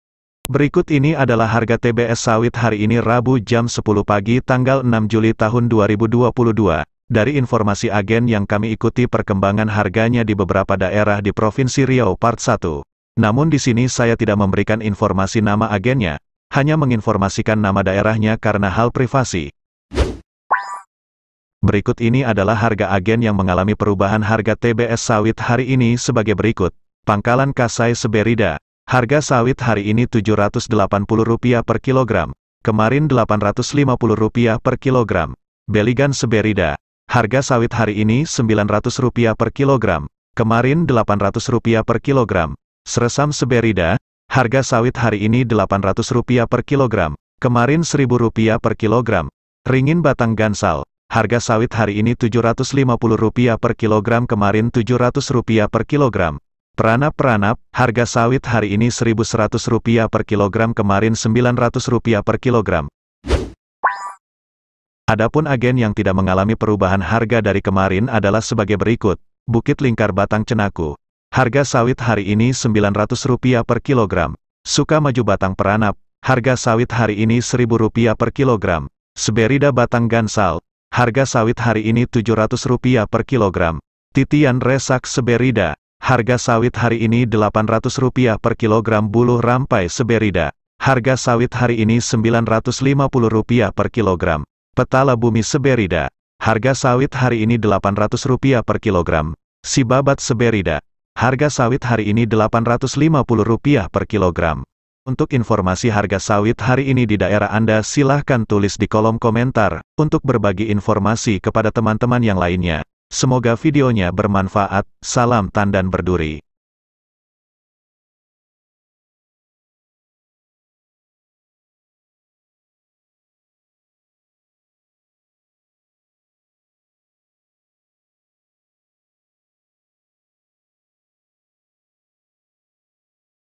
HARGA SAWIT DI AGEN PART 1 - HARGA TBS SAWIT HARI INI DI RIAU 6 JULI 2022
Berikut ini adalah harga TBS sawit hari ini Rabu jam 10 pagi 6 Juli 2022 dari agen yang kami ikuti perkembangan Harganya, di beberapa daerah di provinsi Riau part 1.

Namun Disini Saya Tidak Memberikan Informasi Nama Agennya, Hanya Menginformasikan Nama Daerahnya Karena Hal Privasi.

Berikut Adalah harga agen yang mengalami perubahan harga hari ini adalah sebagai berikut.

Pangkalan Kasai Seberida.
Harga Sawit Hari Ini Rp 780 Per Kilogram.
Kemarin Rp 850 Per Kilogram.

Beligan Seberida.
Harga Sawit Hari Ini Rp 900
Kemarin 800 Per kilogram.

Seresam Seberida.
Harga Sawit Hari Ini Rp 800 kilogram.
Kemarin Rp 1.100 Per Kilogram.

Ringin Batang Gansal.
Harga Sawit Hari Ini Rp 750 Per Kilogram
Kemarin Rp 700 Per Kilogram.

Peranap Peranap.
Harga Sawit Hari Ini Rp 1.100 Per Kilogram
Kemarin Rp 900 Per Kilogram.

Adapun Agen Yang tidak mengalami perubahan harga dari kemarin adalah sebagai berikut..

Bukit Lingkar Batang Cenaku.
Harga Sawit Hari Ini  Rp 900 Per Kilogram.

Suka Maju Batang Peranap.

Seberida Batang Gansal.
Harga Sawit Hari Ini Rp 700 Per Kilogram.

Titian Resak Seberida.
Harga Sawit Hari Ini Rp 800 Per Kilogram

Buluh Rampai Seberida.
Harga Sawit Hari Ini Rp 950 Per Kilogram.

Petala Bumi Seberida.

Sibabat Seberida.
Harga Sawit Hari Ini Rp 850 Per Kilogram.